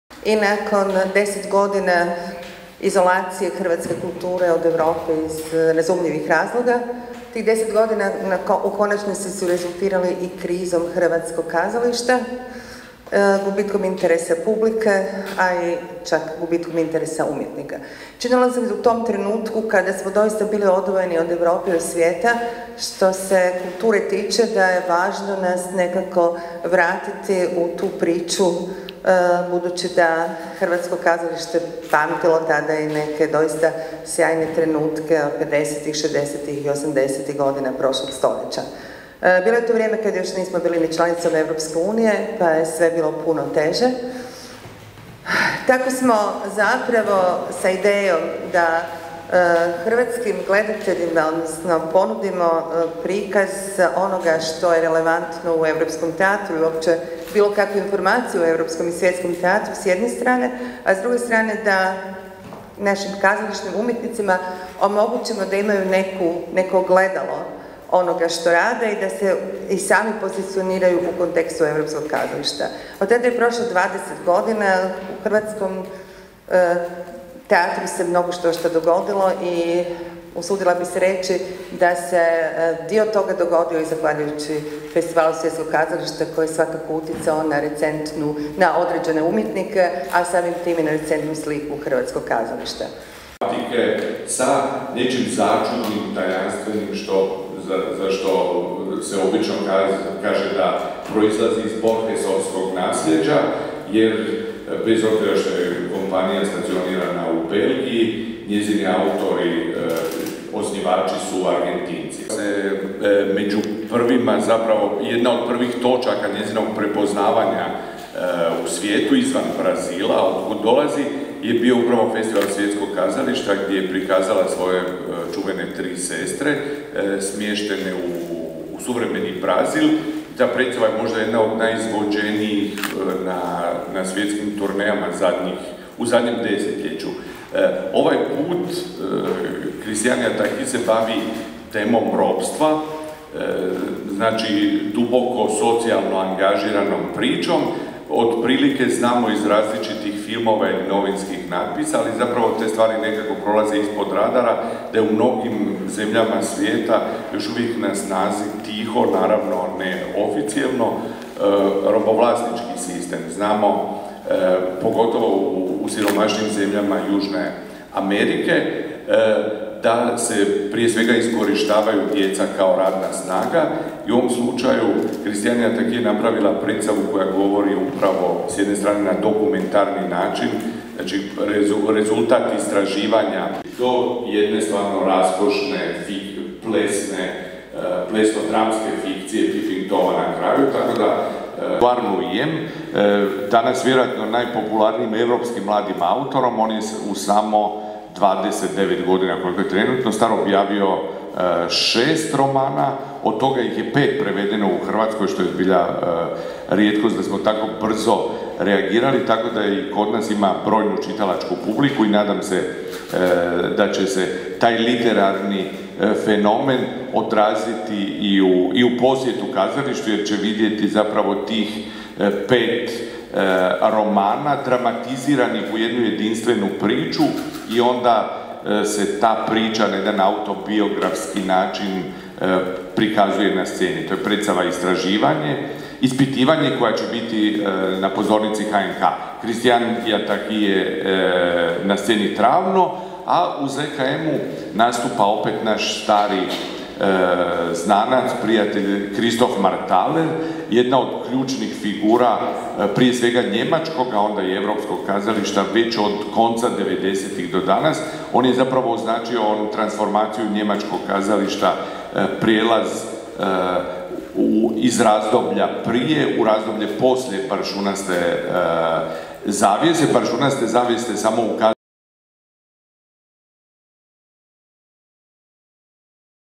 Vrgoč i Buljan najavili Festival svjetskog kazališta
Dubravka Vrgoč i Ivica Buljan najavili su u Hrvatskom narodnom kazalištu Festival svjetskog kazališta koji se održava od 7. do 30. rujna.